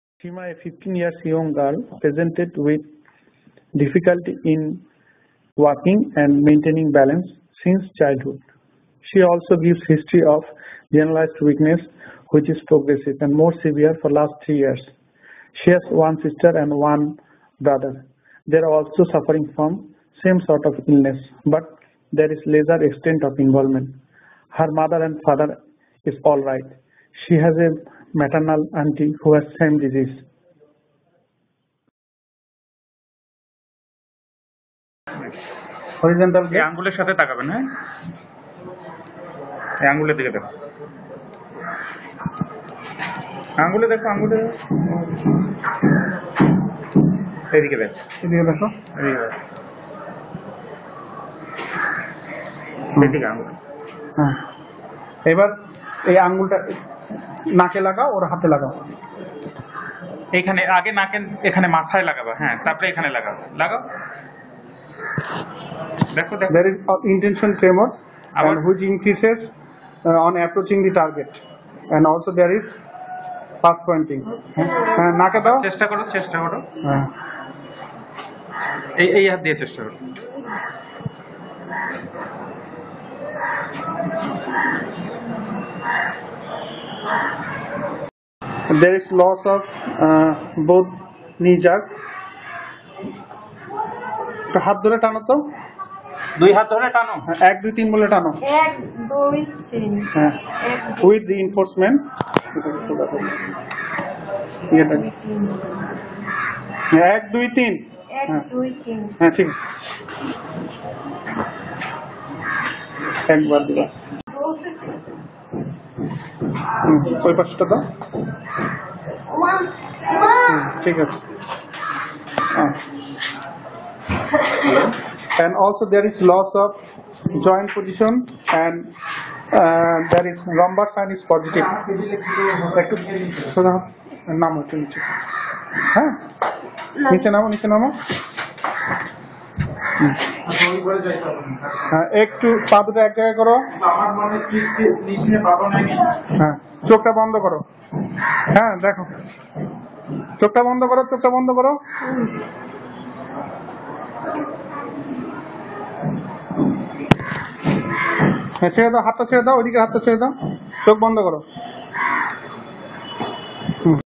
Friedrich's ataxia
Friedrich's ataxia, autosomal recessive inheritence